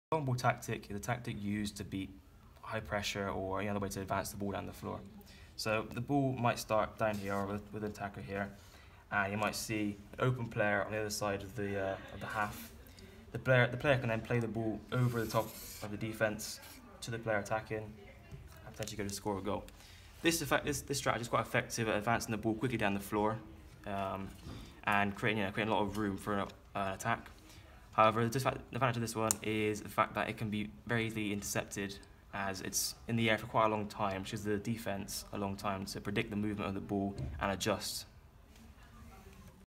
Football long ball
The long ball is a tactic which an be used in the game of football. This tactic is effective at advancing the ball down field quickly. However this pass can be risky as it is easy for the defence to intercept it.